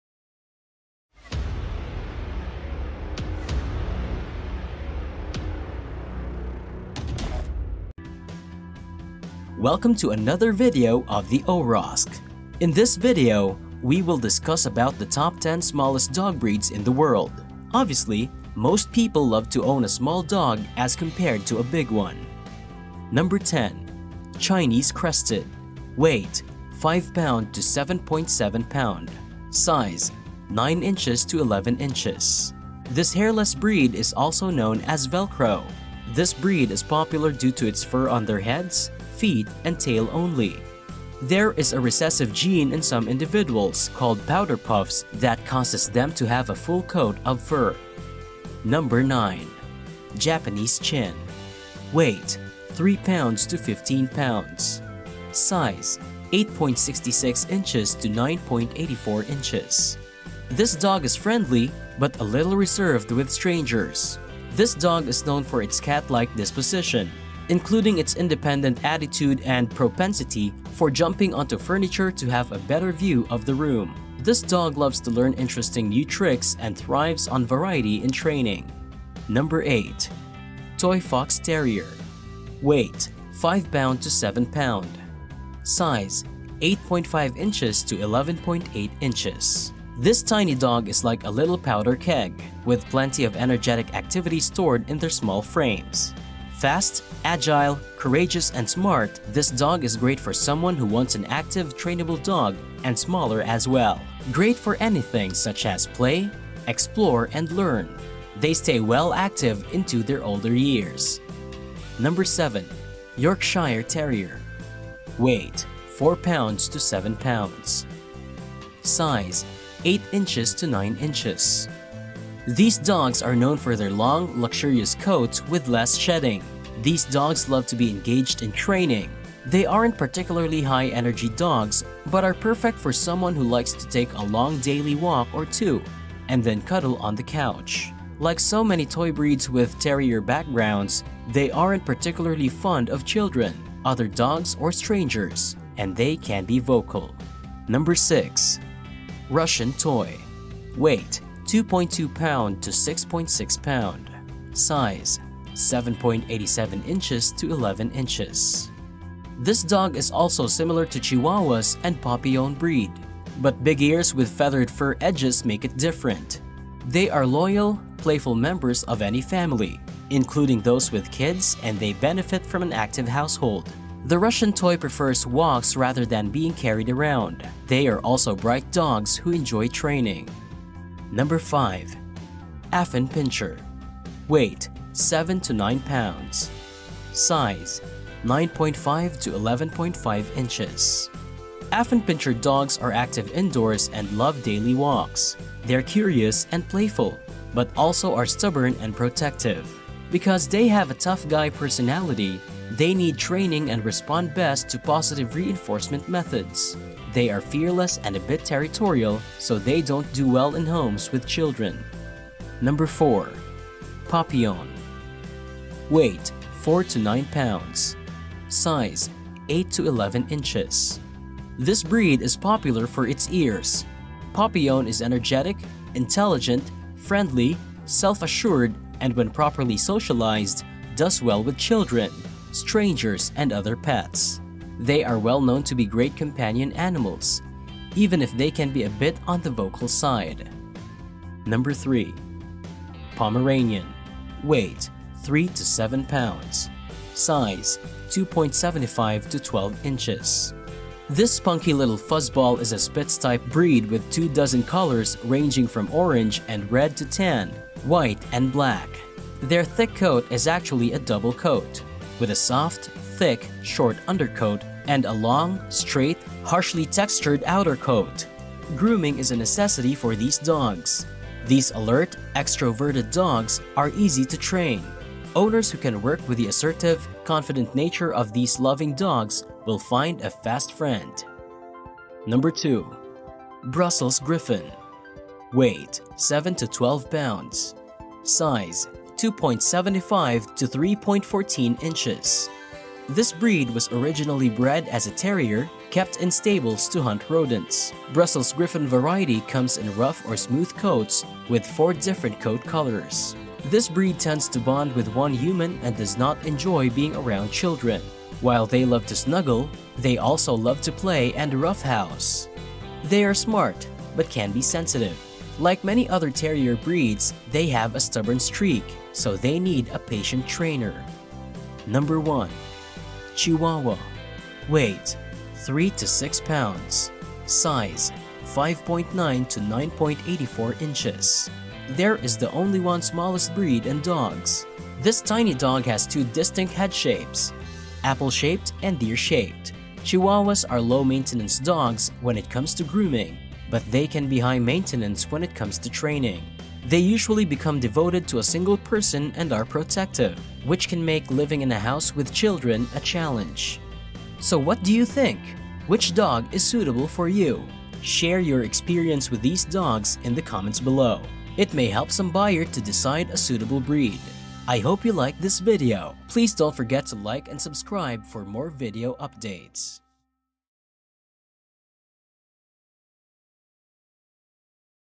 World's Top 10 Smallest Dog Breeds
In this video we will discuss about top 10 Smallest dogs breeds in the world.
Chinese Crested, Japanese Chin, Toy Fox Terrier, Yorkshire Terrier, Russian Toy, Affenpinscher, Papillon, Pomeranian, Brussels Griffon, Chihuahua.